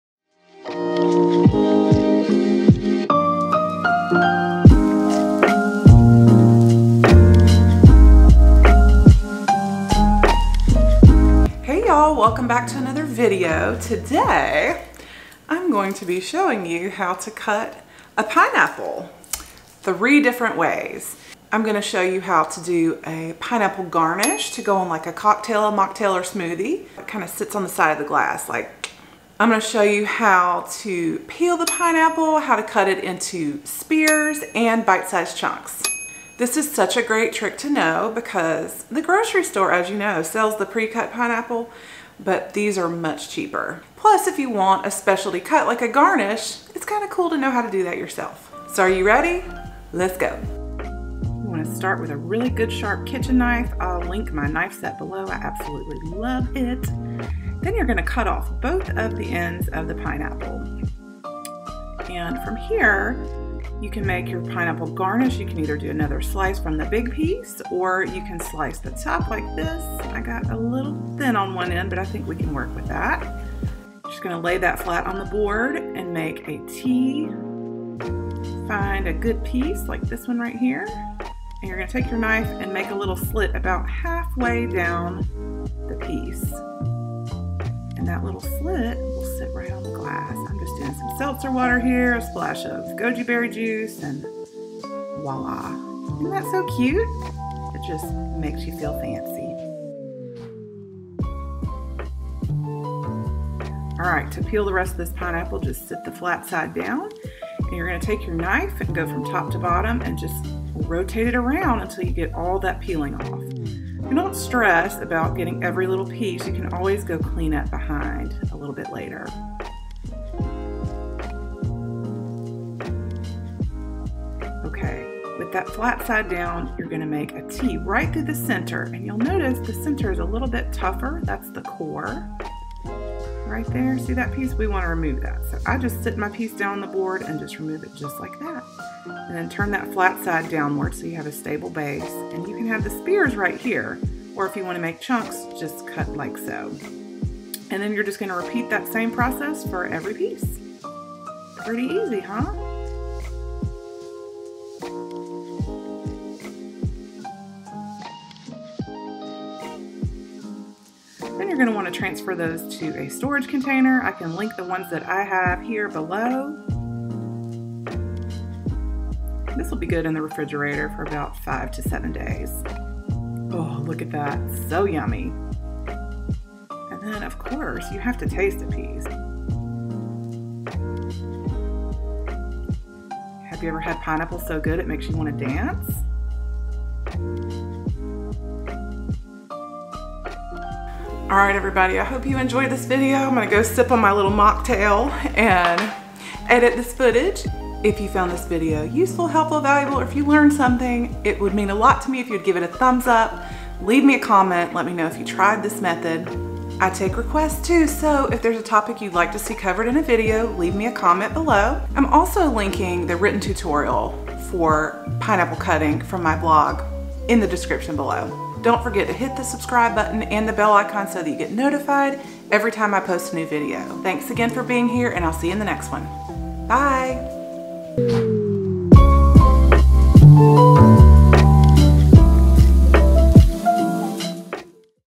HOW TO CUT PINEAPPLE 3 ways: garnish, spears and bite-sized chunks
I will show you how to cut pineapple 3 different ways: a pineapple garnish to sit on a drinking glass, pineapple spears, and bite-sized chunks.

Storage container

Let’s Connect!

Videos shot with Canon EOS M50
Blog photos shot with Nikon D610